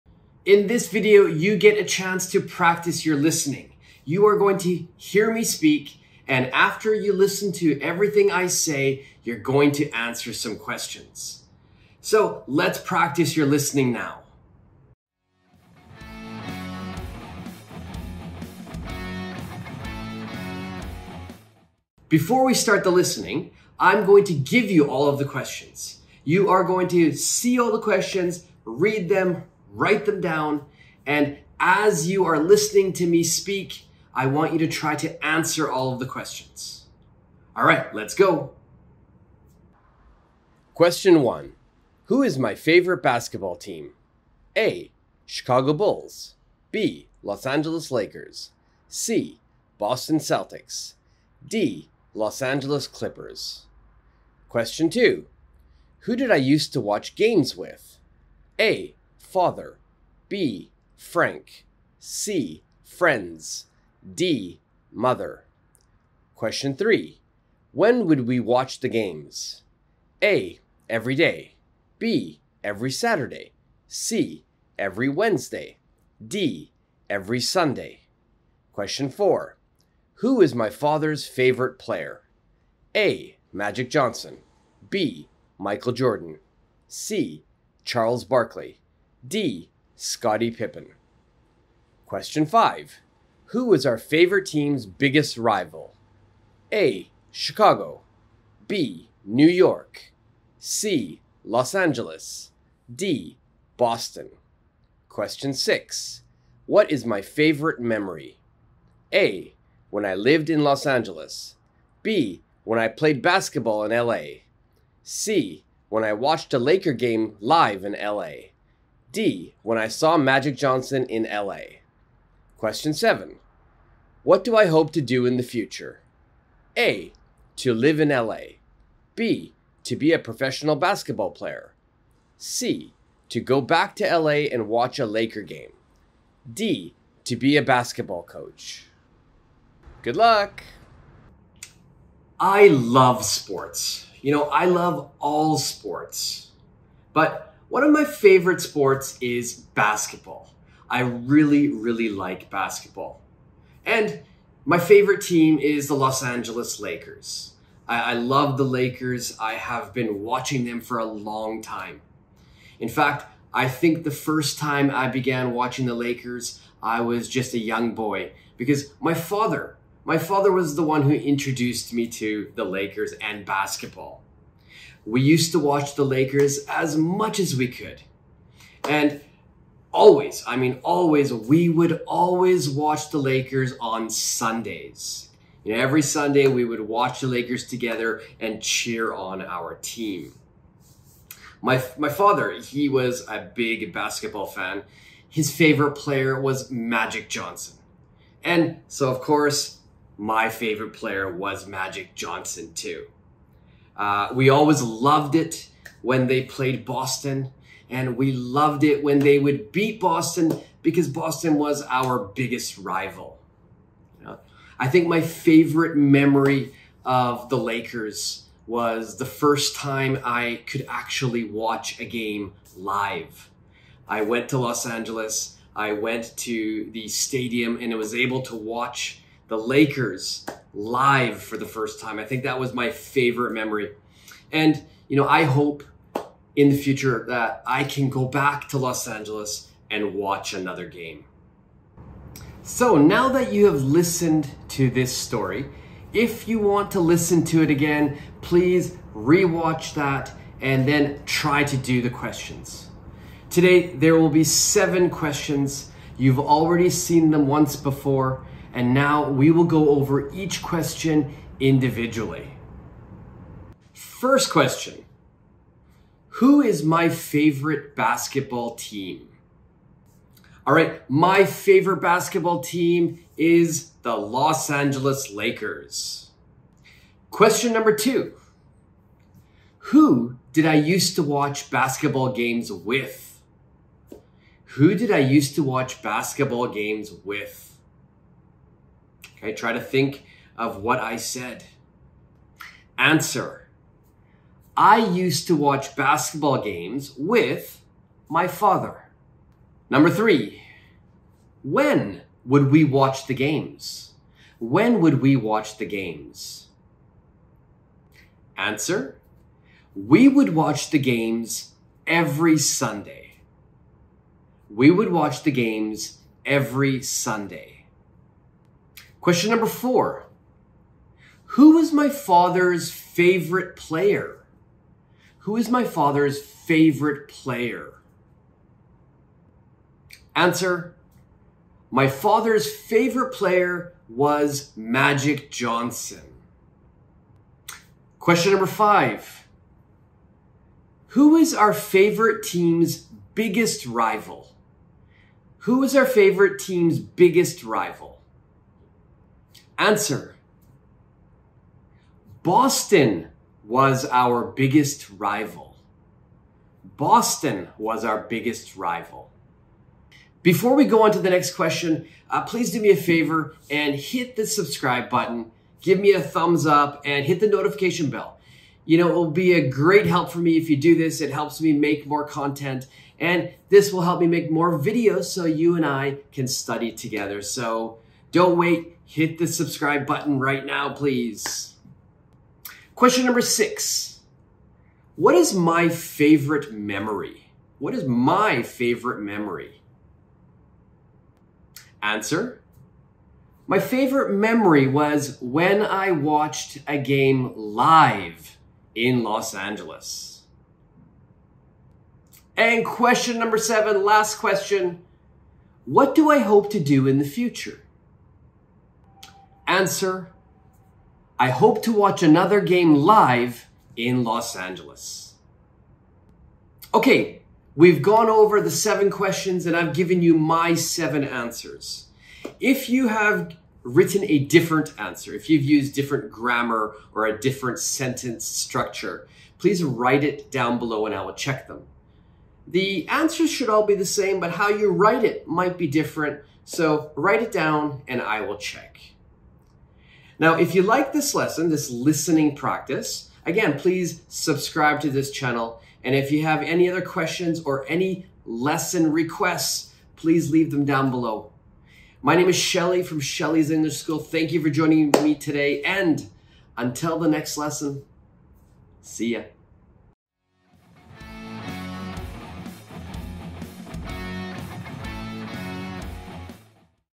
ENGLISH LISTENING & COMPREHENSION TEST!! 👂✍️ 👂 📝 😃 TEST YOUR LISTENING NOW!!!
Do you want to speak like a native English speaker?  If so, you need to train your listening skill as well!!  You can train your listening comprehension skills by watching this lesson. Listen to a story about my life and then test your listening and comprehension by answering 7 questions.

Listen and test yourself now!!!